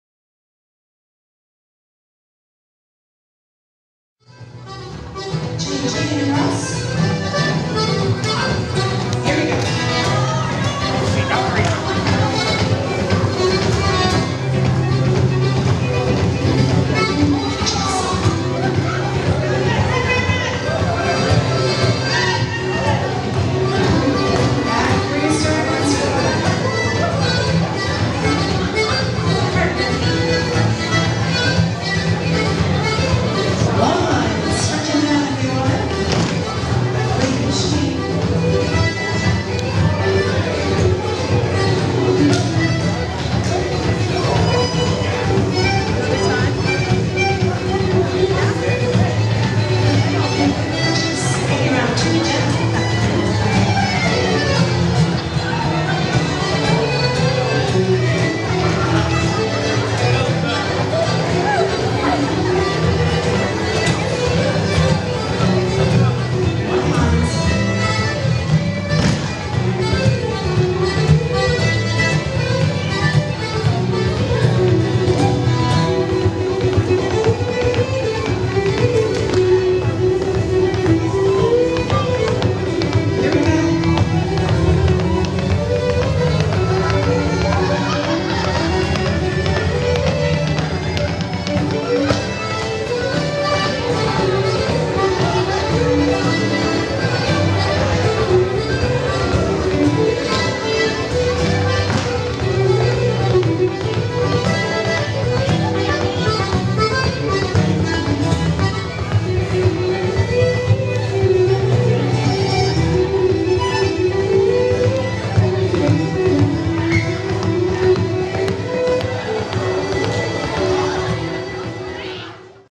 Ottawa Contra Dance March 2015 - Chrissy Fowler & Genticorum
Ottawa Contra Dance MArch 2015 with Chrissy Fowler calling & Genticorum playing.  A lovely fun dance. :)

at the dance as well.